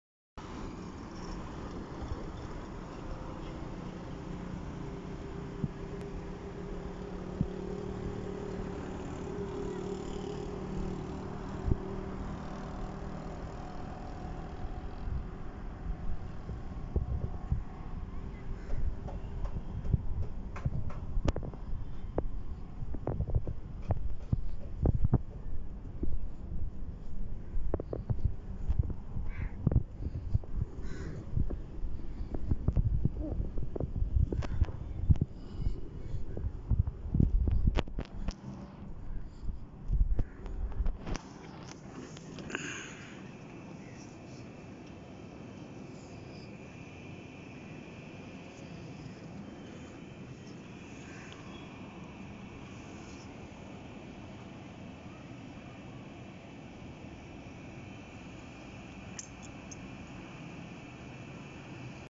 AC Installation in Johar Town
AC Installation in johar Town Near COTHM College Lahore. We provide Best Service with warranty in Lahore City.